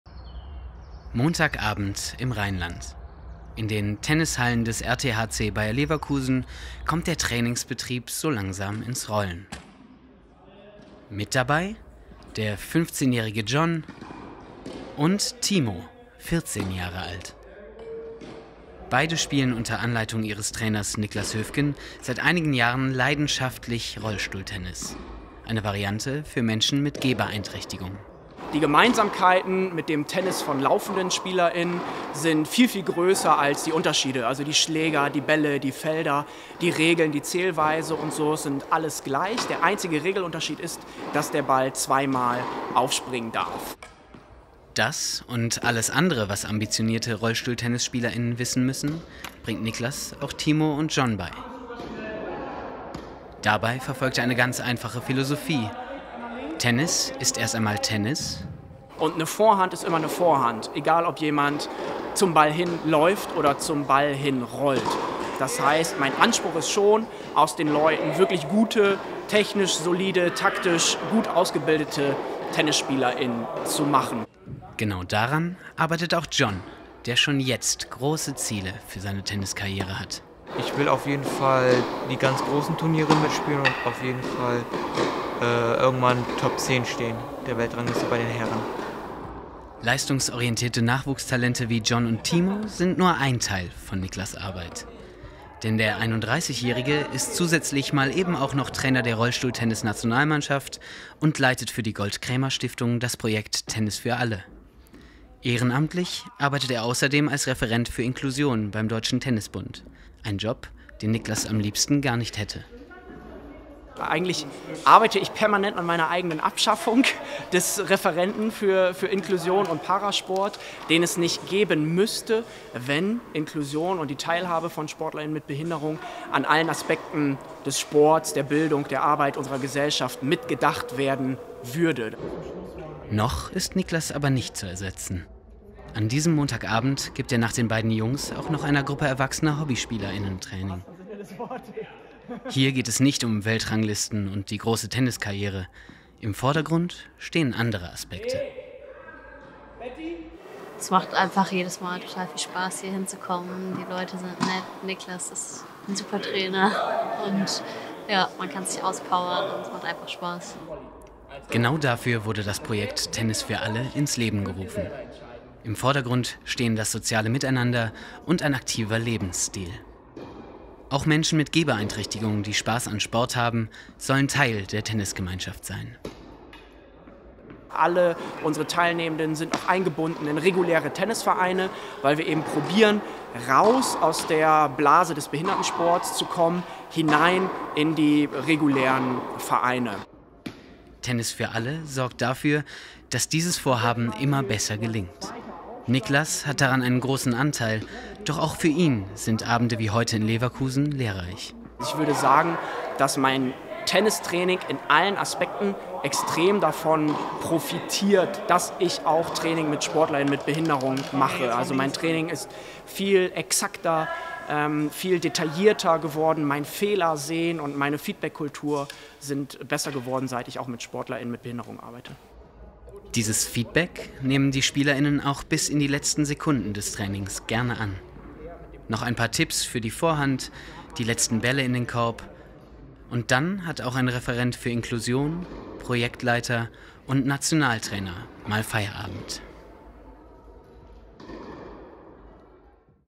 Rollstuhltennis-Training mit Niklas Höfken | DSHS Köln
Besuch beim Rollstuhltennis-Training mit Niklas Höfken vom "Tennis für Alle"-Projekt der Gold-Kraemer-Stiftung, montagabends im RTHC Bayer Leverkusen, Wintersaison 23/24.

Portrait/Reportage (Mrz. 2024) für die Deutsche Sporthochschule von Benedikt Lülsdorf & Jakob Halbfas.